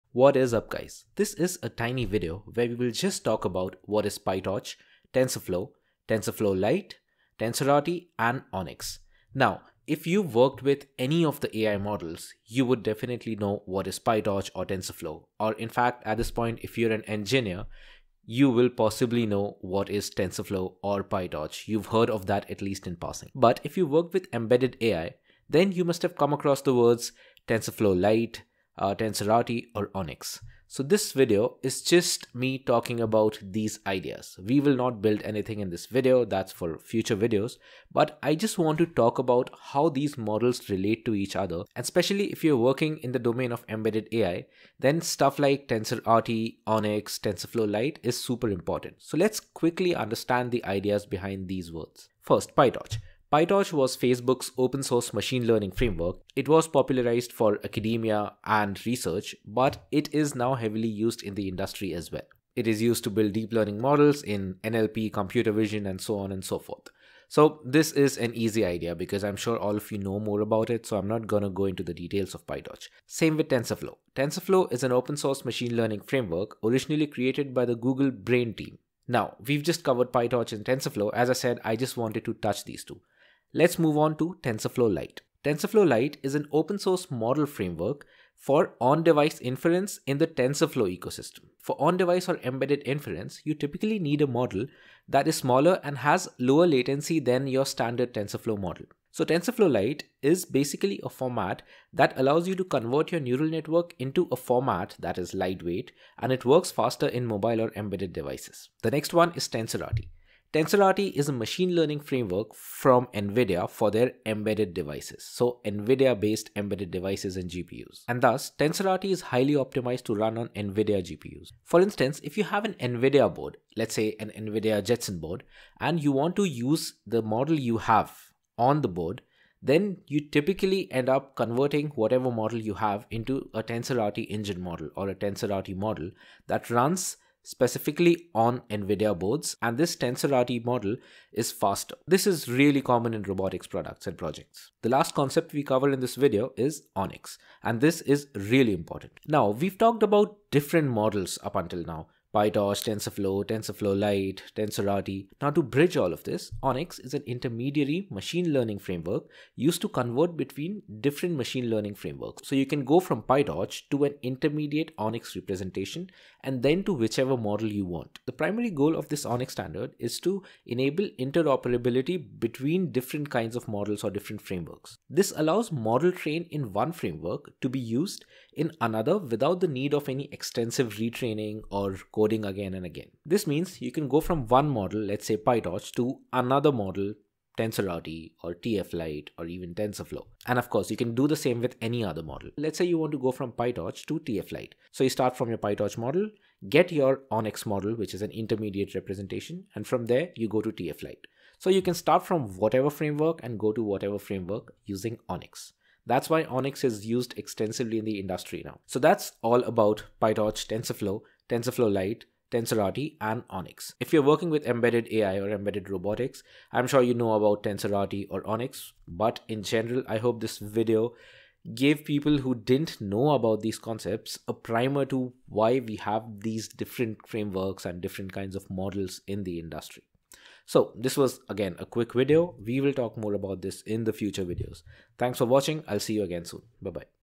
What is Pytorch, TF, TFLite, TensorRT, ONNX?
Basic ideas behind Pytorch, TF, TFLite, TensorRT, ONNX in machine learning